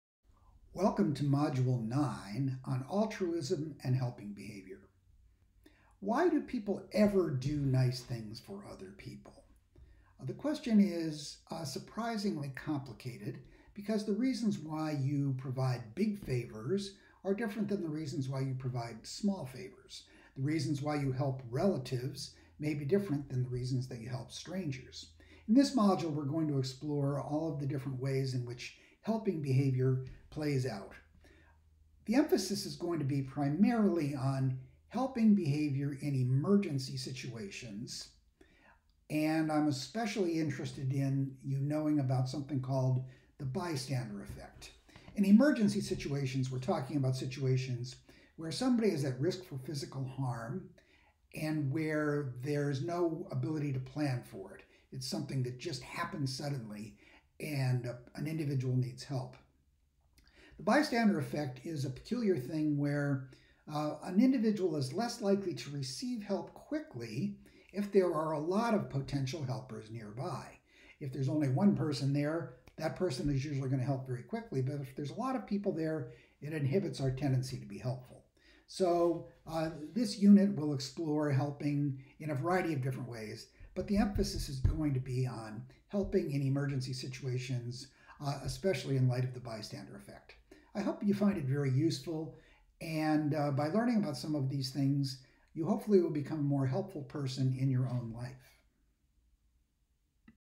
Altruism & Helping Behavior
Knox College Psychology Professor Frank McAndrew introduces the module on altruism and helping behavior for his online social psychology class.